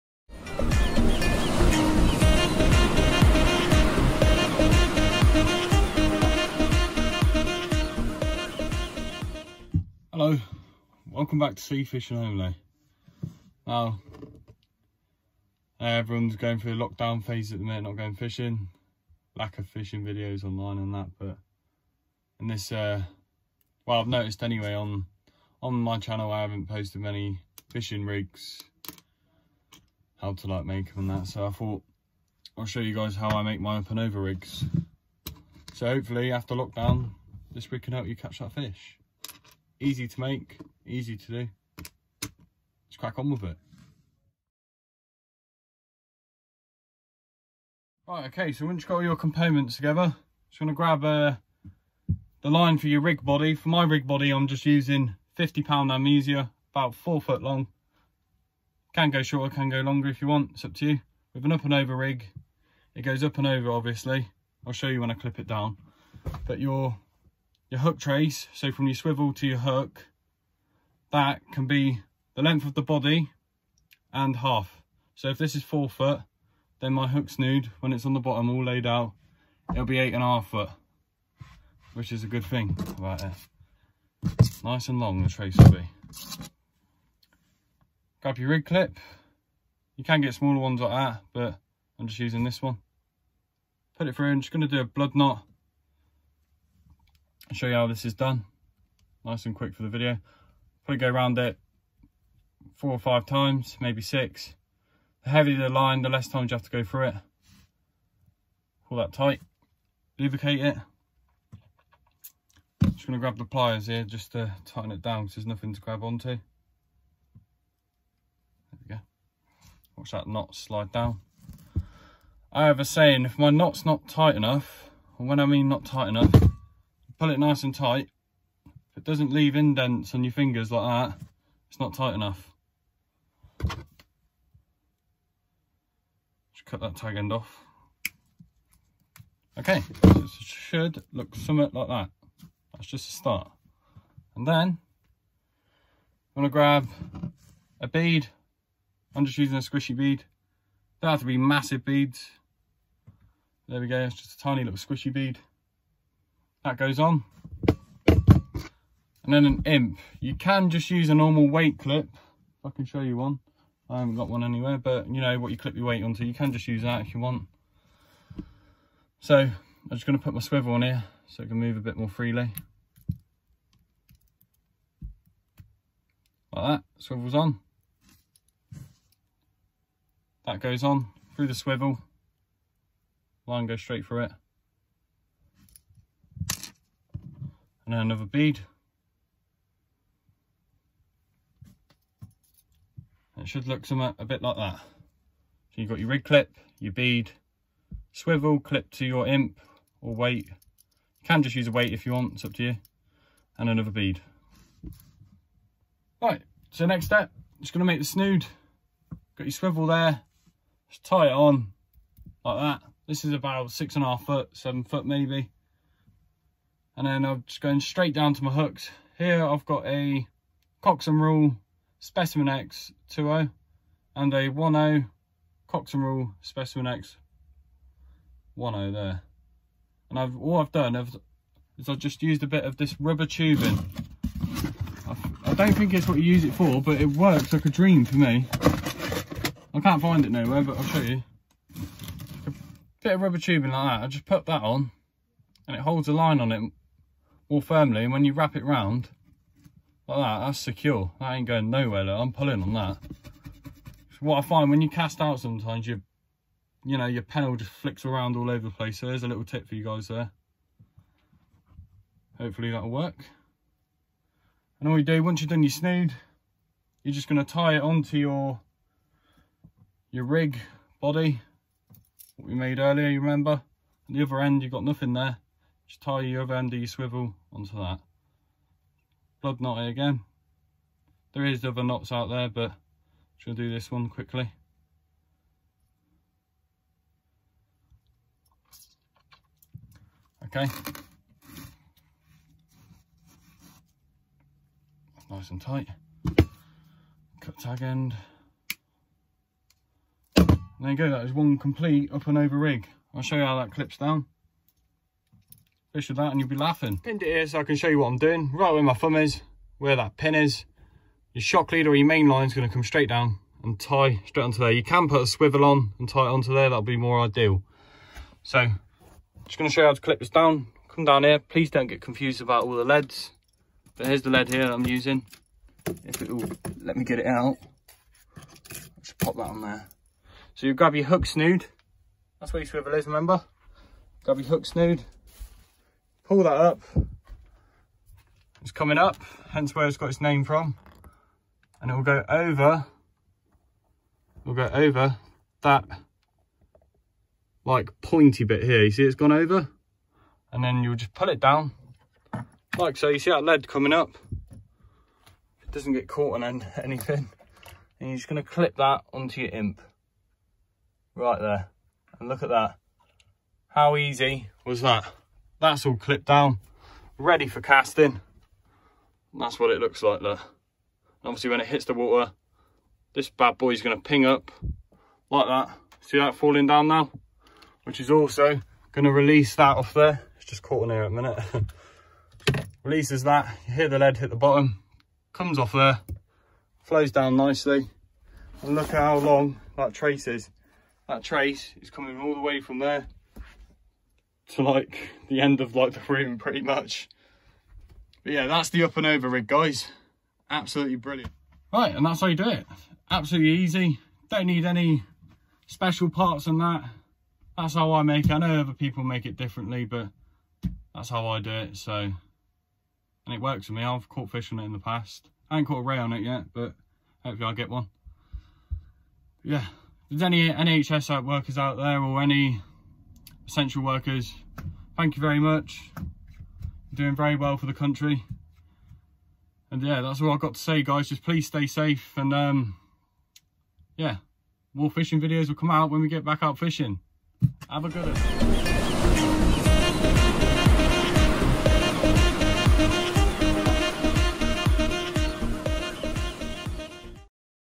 How to make an Up & Over rig
During this lockdown fishing videos have been scarce. This is how I make my up and over rig, hopefully this will help you:)